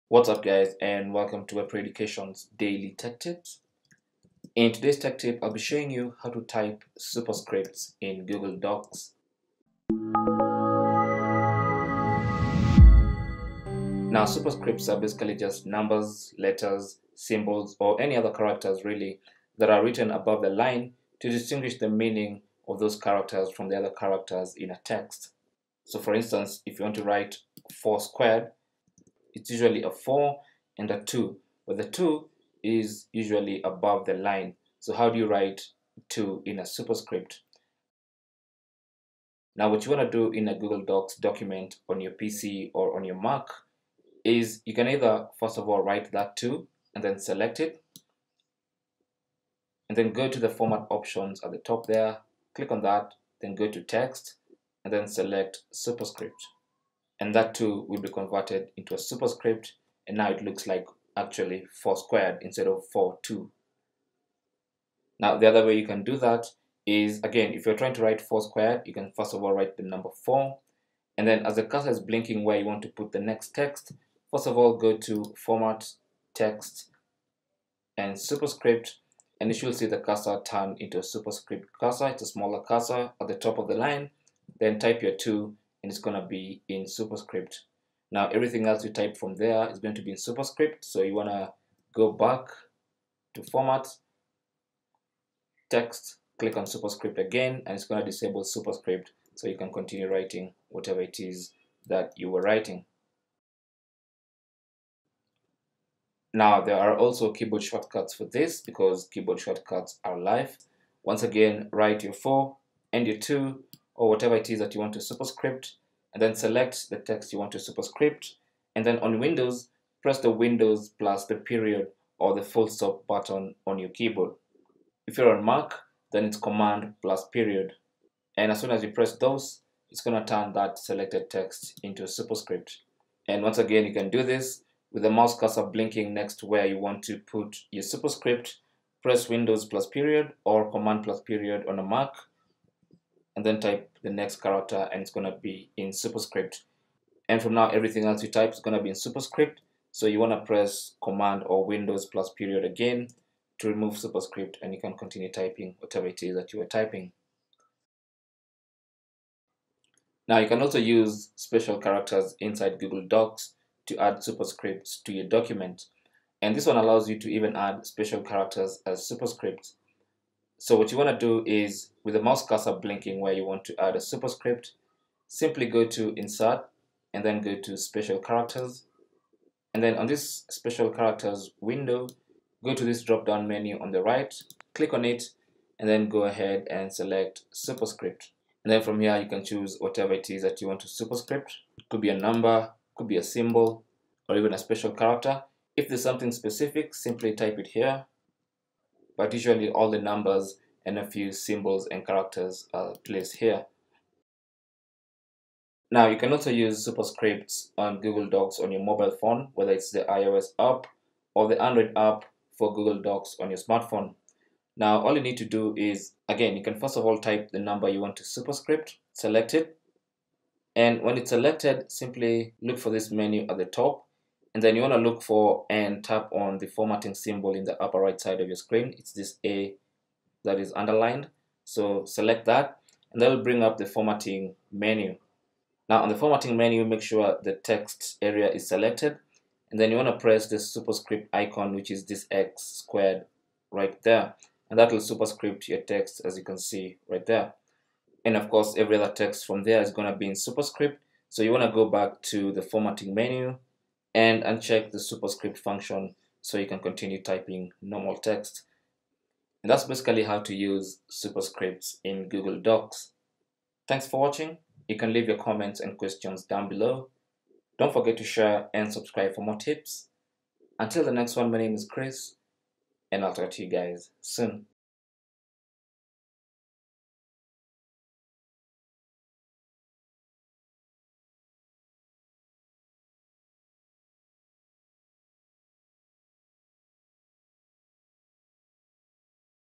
How To Type Superscripts In Google Docs
Learn how to type superscripts in Google docs. A superscript is basically a number, letter or symbol that is written above the line to distinguish it’s meaning from the other characters in a text or document.

From the drop down menu, select ‘Text’ then select ‘Superscript’. That will superscript any selected text. If you had placed the cursor where you want to superscript, then then text type from now on will be superscripted.

Superscript Keyboard Shortcuts In Google Docs:

For windows users, press Ctrl + . and the selected text will be put in superscript format. (.) is sometimes called ‘fullstop’ or ‘period’.
Superscripts In Google Docs

If you’re on a Mac or Macbook, press Command + . to superscript some text.